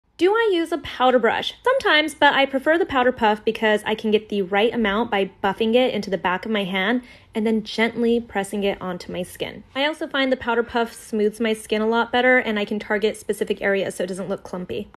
How to use a setting powder: makeup brush or powder puff?
Shout-out to @Makeupbynikkilarose for teaching me the ways of the powder puff. 🫶 Also, I have a few favorite setting powders, but I’m a @LauraMercierCosmetics stan, and always come back to it as my tried-and-true (though the one I used in this video is their newer version). The powder puff is by Kryolan

🔹 Laura Mercier Ultra-Blur Talc-Free Translucent Loose Setting Powder